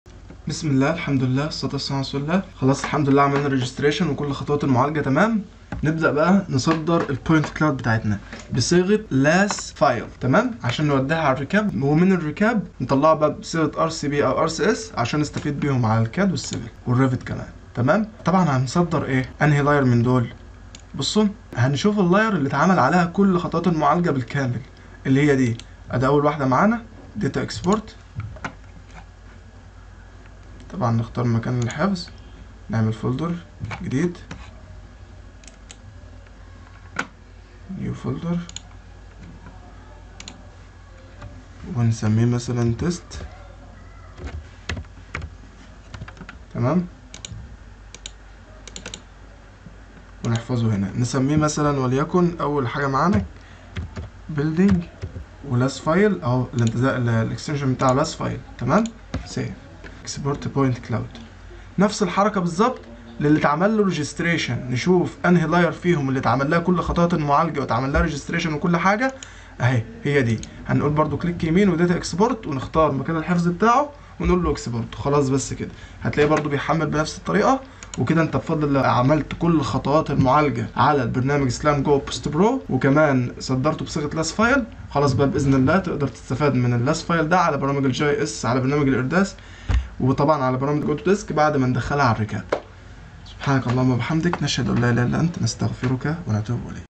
جهاز Slam 100 Laser Scanner // شرح Post Processing // كيفية عمل Export للمشروع
دورة تدريبية متخصصة في تعلم تقنيات المساحة ثلاثية الأبعاد واستخدام جهاز 3D Laser Scanner وتعامل مع تقنيات Geo BIM وتقنيات LIDAR في الأعمال المساحية من إعداد المهندس محمد أشرف

للمزيد من التفاصيل والاستفسارات، يرجى التواصل معنا من خلال الواتساب